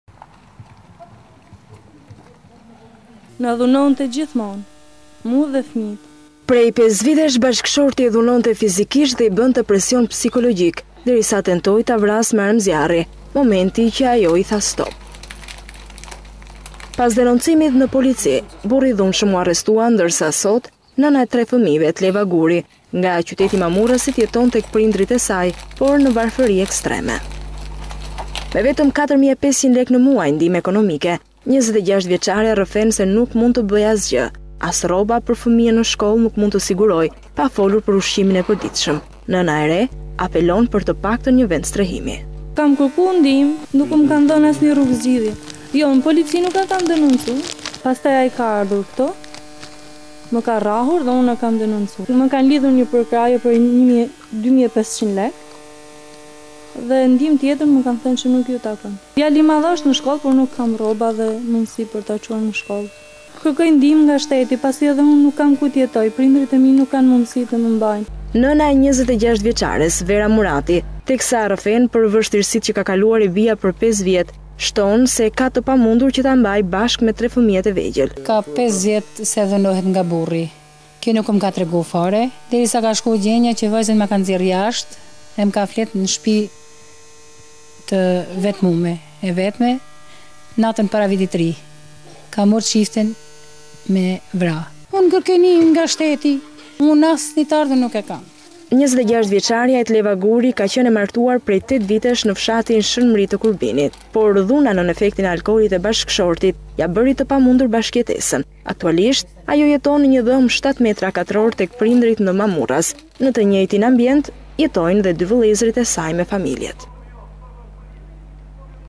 Mamurras, i iku dhunës së bashkëshortit. Jeton në varfëri ekstreme me 3 fëmijë të vegjël
Etleva është vetëm 26 vjeçe, por vuajtjet e 5 viteve martesë, e bëjnë të duket më shumë. Bashkëshorti e dhunonte fizikisht atë dhe 3 fëmijët e saj. I bënte presion psikologjik, derisa tentoi ta vrasë me armë, momenti që i tha stop!
“Na dhunonte gjithmonë. Mua dhe fëmijët” tregon Etleva ndërsa në prehër mban djalin e vogël.
Pas denoncimit në polici, burri i dhunshëm u arrestua, ndërsa sot nëna e tre fëmijëve Etleva Guri nga qyteti i Mamurrasit jeton te prindërit e saj, por në varfëri ekstreme.
Me vetëm 4500 lekë në muaj ndihmë ekonomike, 26 vjeçarja rrëfen, se nuk mund të bëjë asgjë. As rroba për fëmijën në shkollë nuk mund të sigurojë, pa folur për ushqimin e përditshëm. Nëna e re apelon për një vendstrehim.